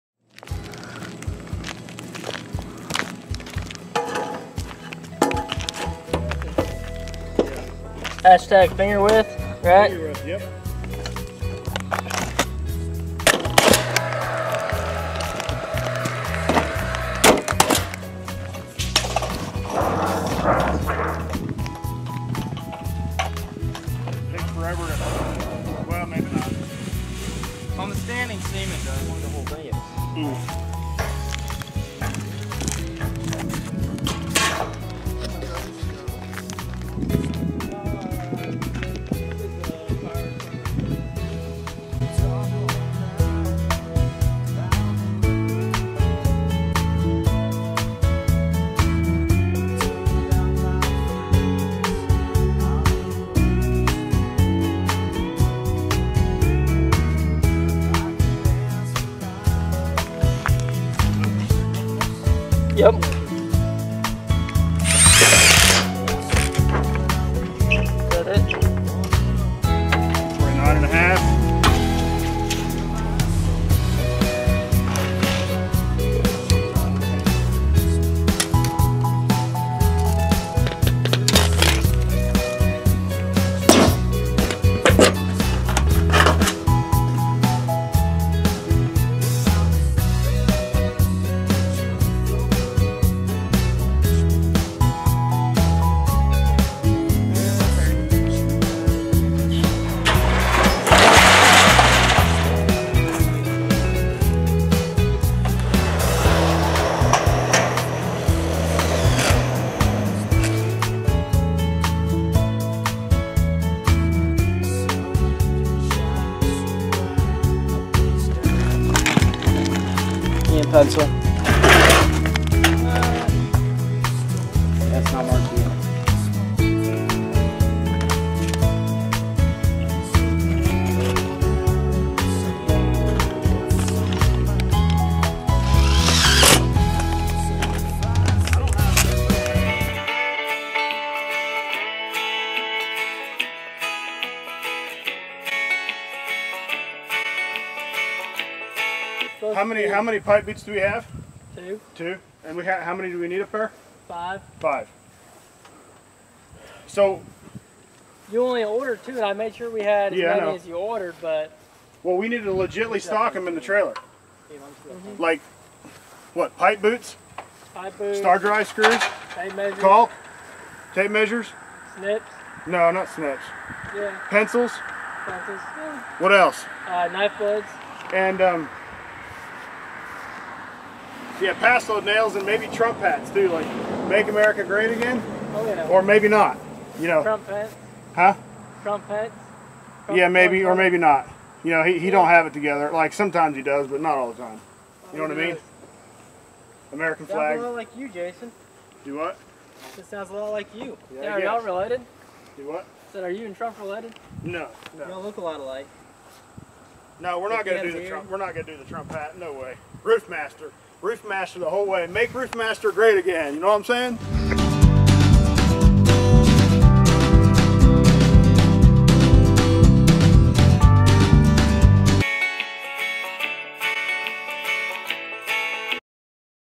Mennonite Working on a Metal Roof in Kentucky - Why a Metal Roof
I got to shot this film of some Mennonites working on a metal roof in the state of Kentucky. I like here were they show us a few tips and tricks on how to make there working more effectively ! ohh needless to say they support trump ..

Montez McCamish